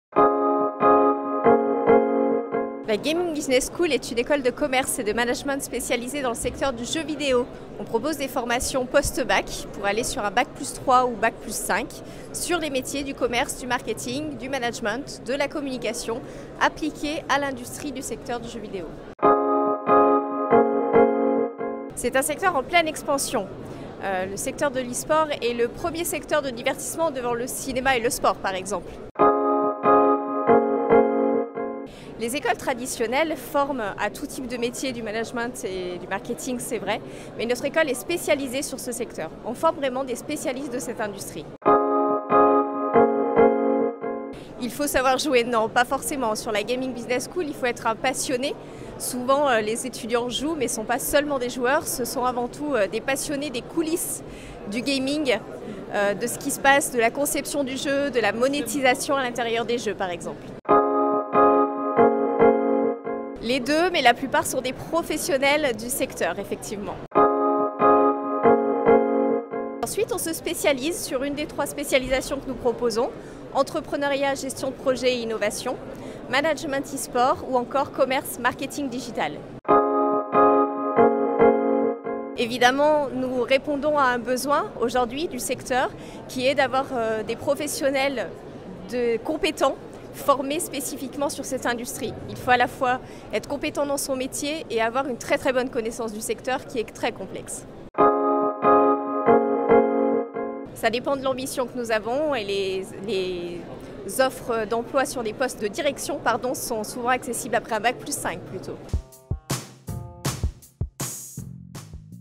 Gaming Business School : tout ce que vous devez savoir
StudyramaTV. Karen Faucon-LoPinto, responsable coordination de la Gaming Business Schooll vous explique tout ce qu'il faut savoir sur l'école et le développement des métiers liés aux métiers du jeu vidéo, du gaming, de l'e-sport...